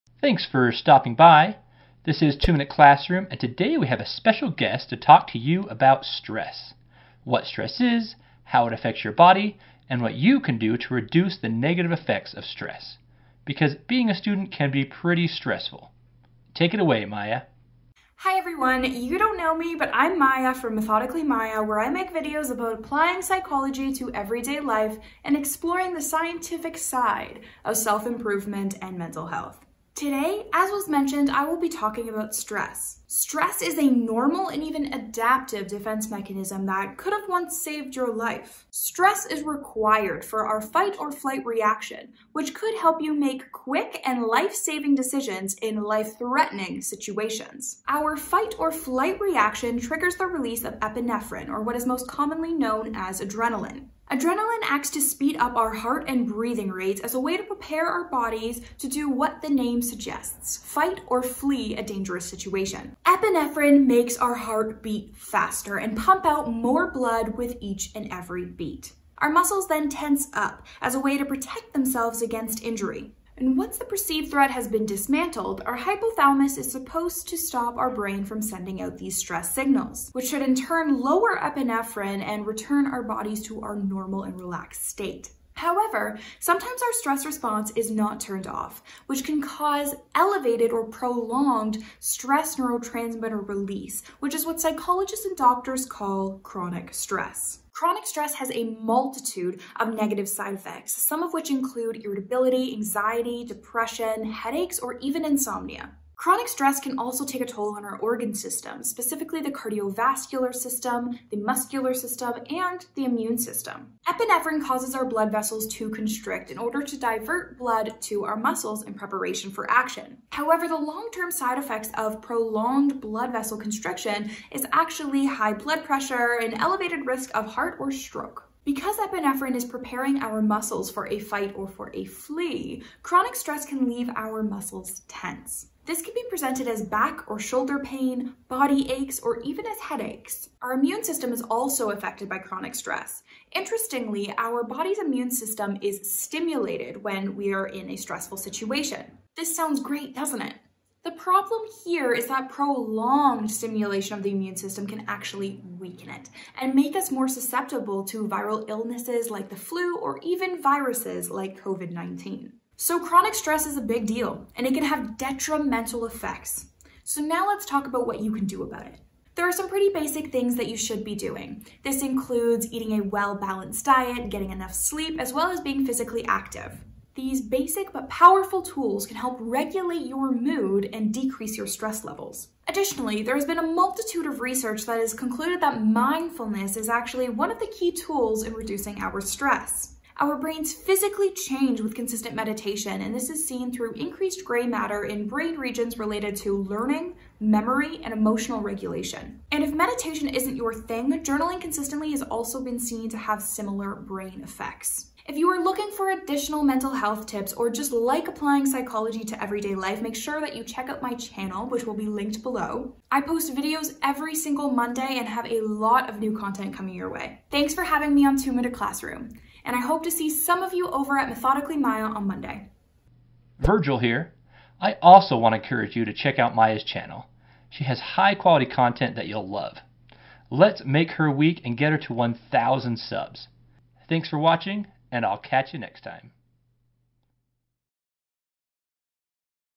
How Stress Affects Your Body and How You Can Reduce Stress | Featuring Methodically Maya
---TRANSCRIPT---
Thanks for stopping by, this is 2 Minute Classroom and today we have a special guest to talk to you about stress. What stress is, how it affects your body, and what you can do to reduce the negative effects of stress.

Because being a student can be pretty stressful.

Take it away Maya!

Hi everyone, you don’t know me but I’m Maya from Methodically Maya where I make videos about applying psychology to everyday life and taking a scientific approach to self-improvement & mental health.

Today, as was mentioned, I will be talking about stress.

Stress is a normal and even adaptive defense mechanism that historically could have saved your life. Stress is required to trigger the body’s fight or flight reaction which could help you make quick and life-saving decisions in potentially dangerous situations.

Our fight or flight reaction triggers the release of epinephrine, or what is more commonly known as adrenaline, which acts to speed up our heart and breathing rates to prepare the body to do what the reaction’s name suggests, fight or flee the threatening situation.

Epinephrine makes our hearts beat faster and pumps more blood out with each and every beat as a way to prepare our muscles for action. Our muscles then tense up in an act to protect themselves from injury when having to deal with the threat.

Once the perceived threat has been dismantled, our hypothalamus should tell our central nervous system to stop releasing epinephrine which should return the body to its normal, relaxed state.

However, in some cases, our stress response doesn’t get turned off and our brains release stress neurotransmitters for longer than needed and this is what psychologists and doctors would call chronic stress.

Chronic stress has a multitude of negative side effects some of which include: irritability, anxiety, depression, headaches, and insomnia. Chronic stress can also take a toll on your organ systems: specifically, the cardiovascular system, the muscular system, and the immune system can all be greatly affected.

Epinephrine causes your blood vessels to constrict in order to divert blood to the muscles in preparation for action; however, the long-term side effects of this include elevated blood pressure leading to an increased risk of stroke or heart attack.

Because epinephrine is preparing our muscles for a fight or for a flee, chronic stress leaves our muscles tense. This can lead to muscle tightness which may present itself as back or shoulder pain, body aches, or even as headaches.

Our immune system is also affected by chronic stress. Stress will actually stimulate the immune system as a way to prepare for the healing of wounds or even as a way to avoid infections. This sounds great, doesn’t it? However, over time this prolonged stimulation acts to weaken our immune system and can make us more susceptible to viral illnesses like the flu, the cold, or even viruses like COVID-19.

So chronic stress is a big deal - and can have detrimental effects. Now let’s talk about what you can do about it.

There are some pretty basic things that you want to make sure you are doing - this includes getting enough sleep, eating a healthy and balanced diet, as well as being physically active. These basic but powerful tools will help to regulate your mood and manage your stress levels.

Additionally, there is a multitude of research that points to mindfulness as being one of the key tools used to reduce stress and improve many cognitive and behavioral functions. Your brain physically changes with consistent meditation by increasing the grey matter concentration in brain areas relevant to learning, memory, and emotional regulation. If meditation isn’t your thing, journaling consistently has been seen to have similar mental health effects.

If you are looking for additional mental health tips or if you like applying psychological principles to everyday life, then make sure that you check out my channel which will be linked below. Thanks for having me 2 Minute Classroom, and I hope to see some of you over at Methodically Maya on Monday.

Virgil here.

I also want to encourage you to check out Maya’s channel. She has high-quality content that you’ll love.

You all rock.